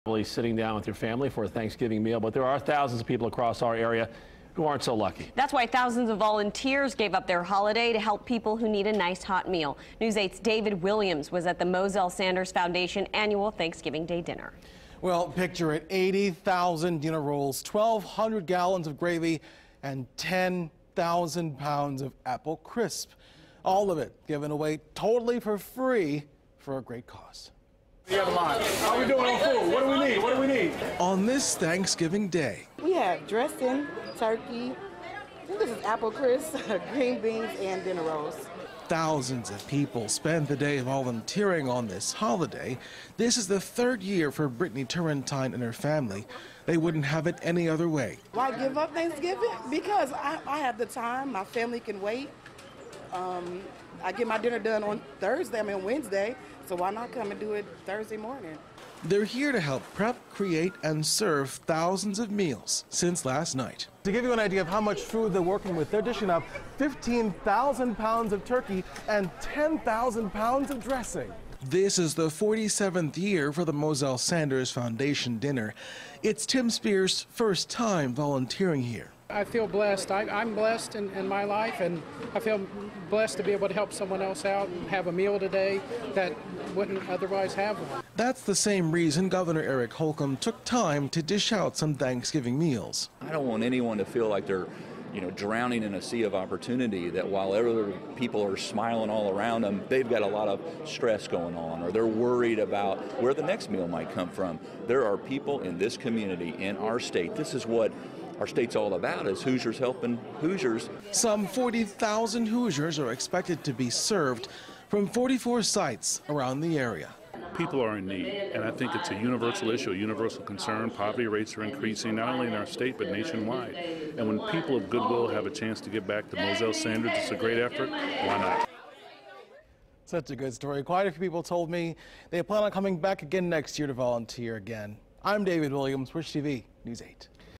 Thanksgiving dinner served by thousands of volunteers
News 8's David Williams reports.